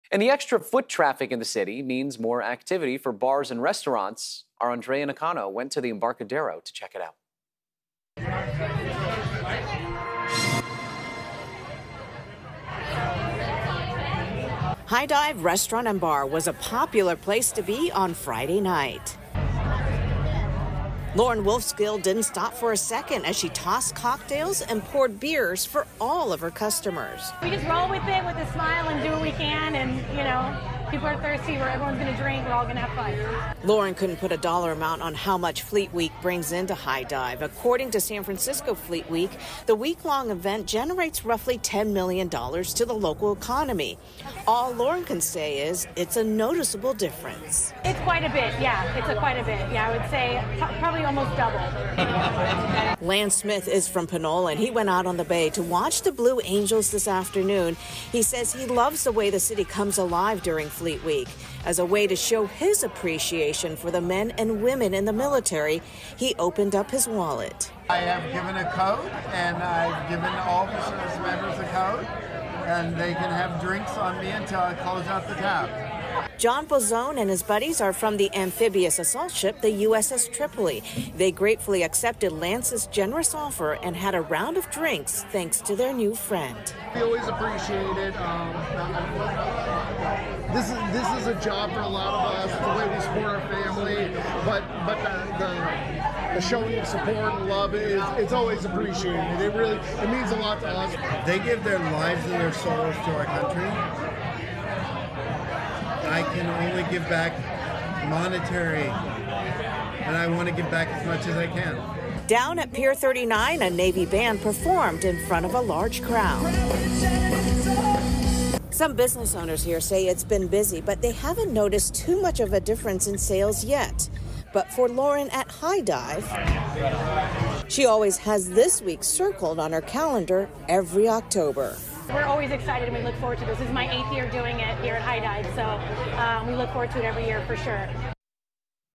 San Francisco bars see boost from Fleet Week crowds
Andrea Nakano reports on the increase in business San Francisco bars and restaurants are seeing thanks to Fleet Week foot traffic.